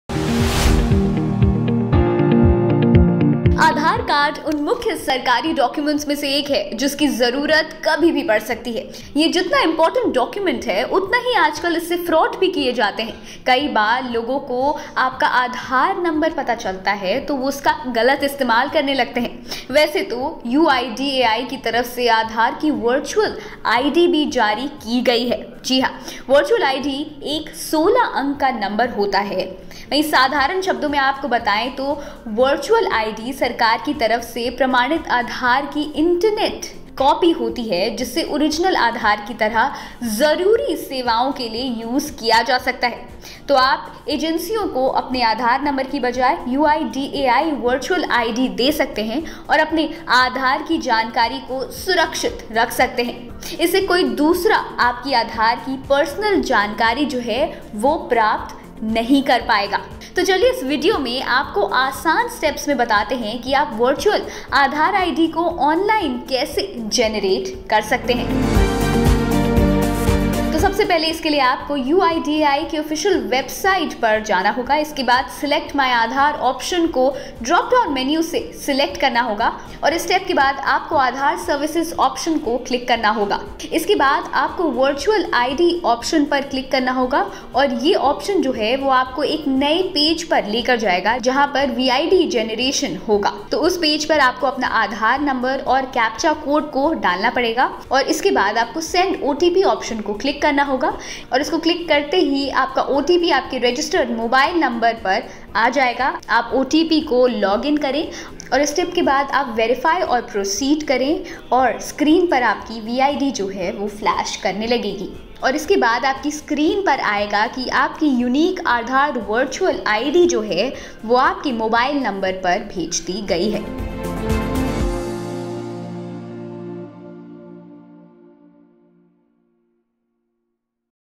How to generate virtual ID using Aadhaar | Step by step explanation | Tutorial | Aadhaar update
An Aadhaar Virtual ID has been implemented by UIDAI in 2018. It is a temporary, revocable 16-digit random number generated against an Aadhaar number. The ID remains valid till the next Aadhaar Virtual ID is created.